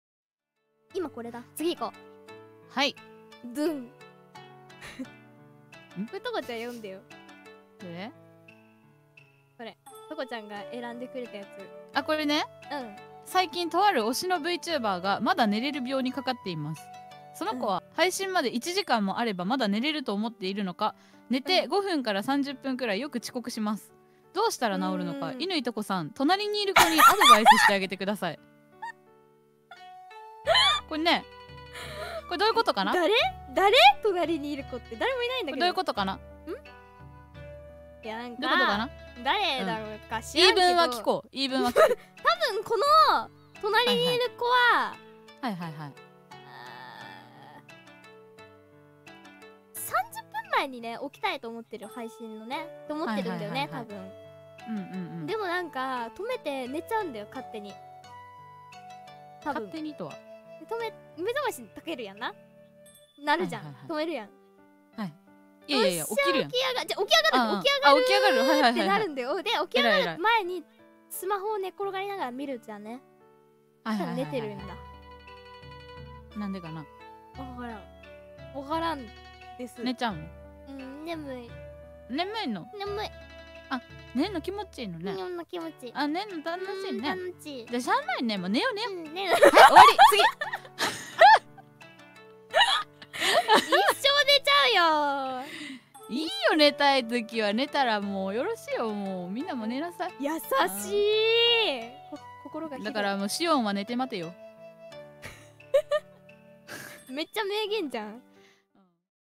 Murasaki Shion, Nap Queen
I don't know who they're talking about, but she's way past the point of no return.

Aired on June 12, 2019.

QC: Me
Thumbnail is a random frame of the clip.